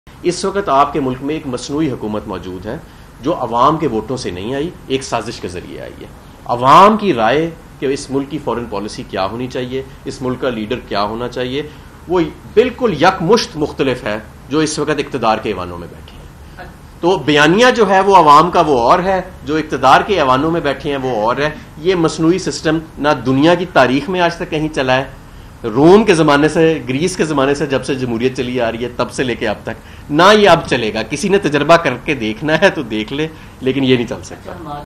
"Iss Waqt Mulk Mai Ek Masnoee Hukumat Maujood Hai" PTI Rehnuma Hammad Azhar | Dawn News
"Right Now There Is An Artificial Government In The Country" PTI Leader Hammad Azhar | Dawn News

'اس وقت ملک میں ایک مصنوعی حکومت موجود ہے'

Infocus:
shorturl.at/bmrHU

Yes Chef Mehboob:
shorturl.at/qrxG1